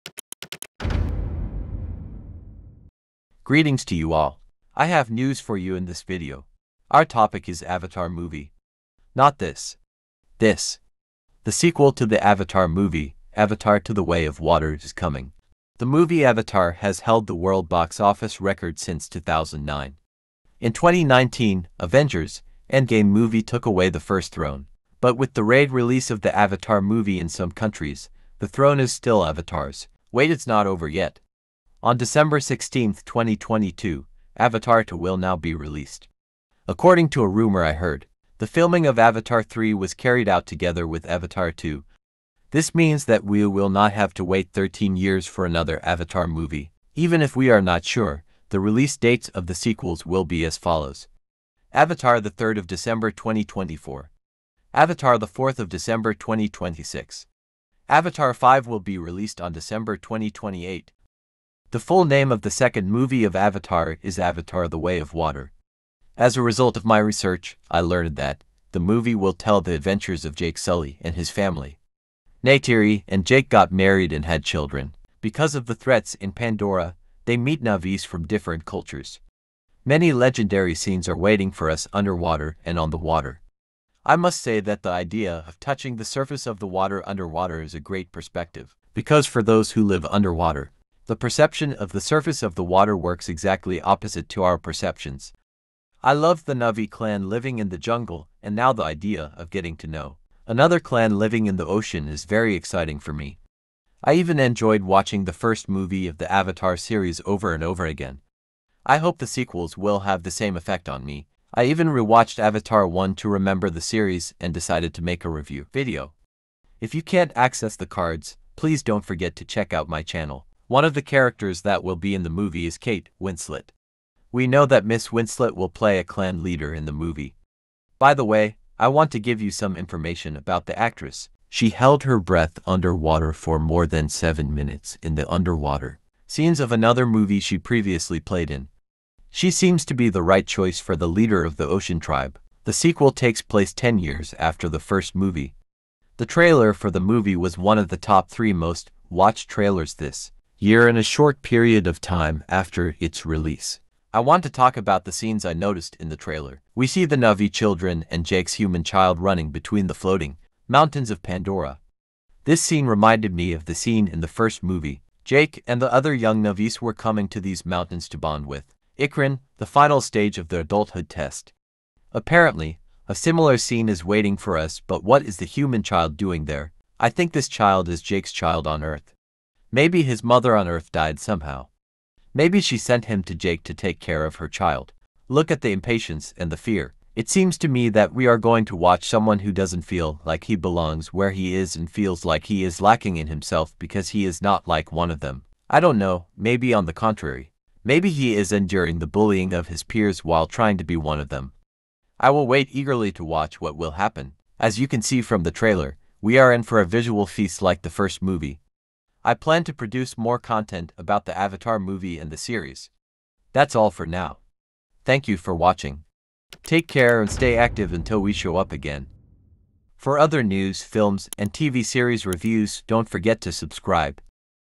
AVATAR 2 | THE WAY OF WATER (2022) News & Trailer Review
greetings to you all. I have news for you in this video. Our topic is avatar movies. not this. this. The sequel to the Avatar movie, Avatar 2 the Way of Water is coming.
The movie Avatar has held the world box office record since 2009. In 2019, Avengers: Endgame movie took away the 1st throne, but with the re-release of the avatar movie in some countries, the throne is still Avatar's. wait it's not over yet. On December 16, 2022, avatar 2 will now be released.